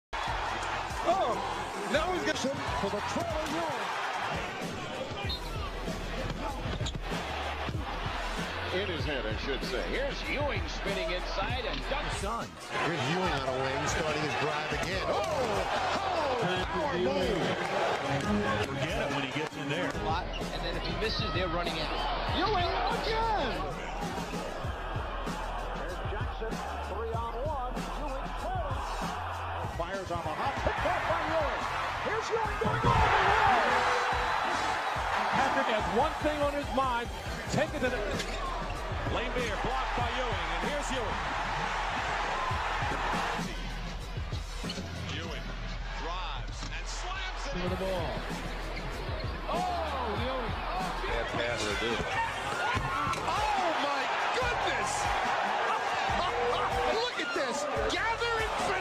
Patrick Ewing was a Beast in his Prime
Some of Patrick Ewings Most Iconic Plays